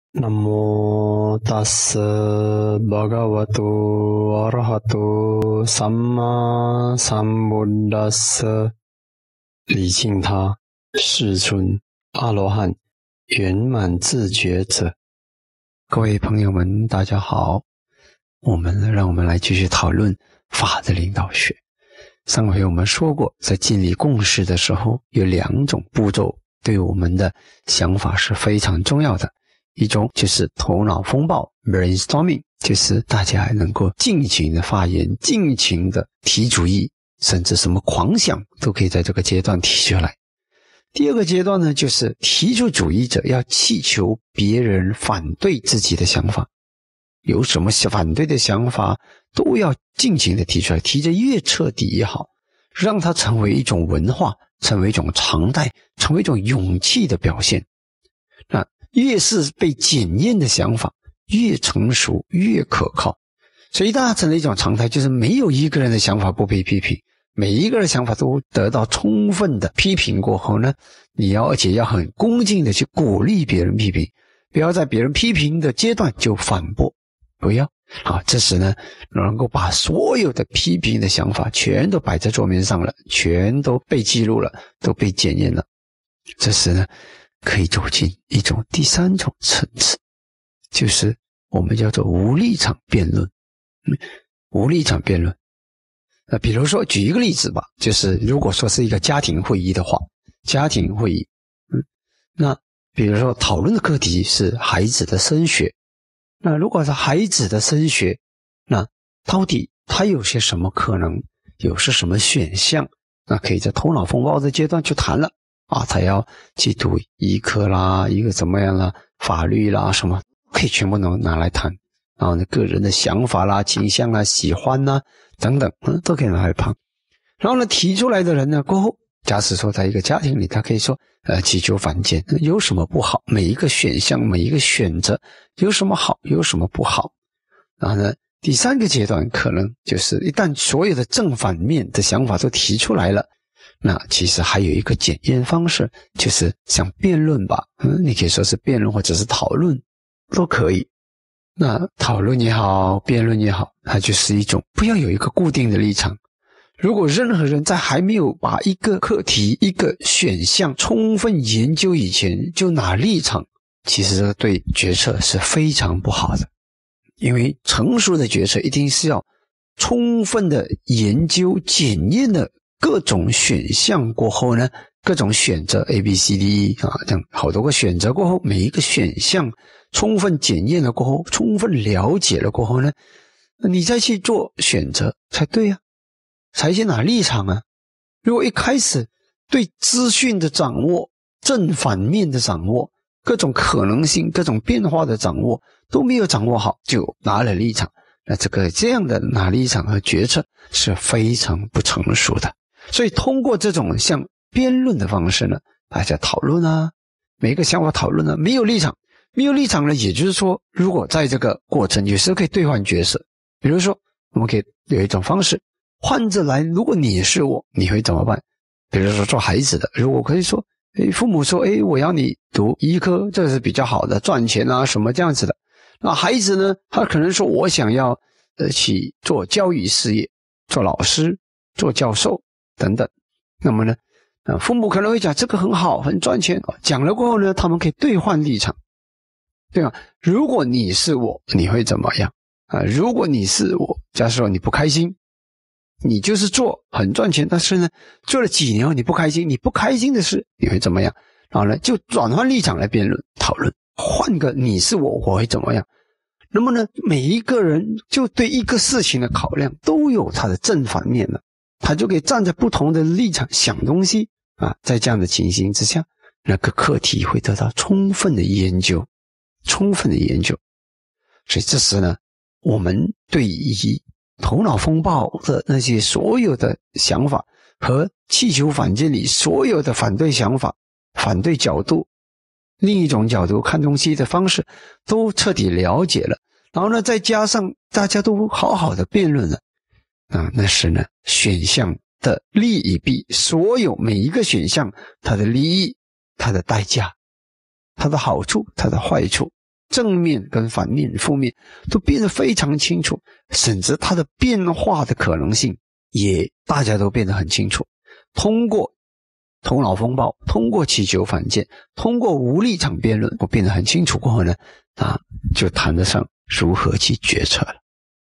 「法的领导学」系列之十九：无立场辩论
📻一个成熟可行的想法、点子、决策或方案，都需要经过全方位的考量和检测。无立场辩论能逼使自己可以站在不同的视角和维度来看待问题，搜集与参考相关的资讯和数据，无死角地把一切正反利弊和变量都摆在台面，以便能够在方方面面的知情下作出最好的决策。欢迎大家恭听吉祥尊者分享法的领导学，善用辩论来拓展视野和思路，继续从光明走向光明。☸